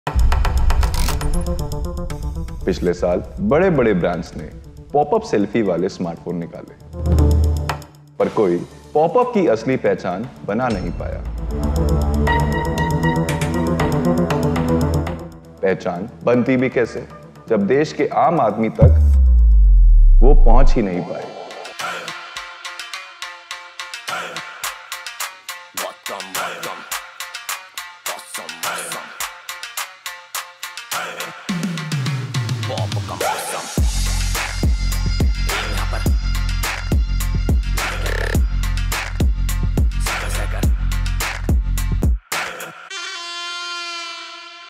Infinix S5 Pro | Official Trailer | 6 March
The much awaited S5 Pro with Pop-up selfie camera will be launched on the 6th of  March at a killer price.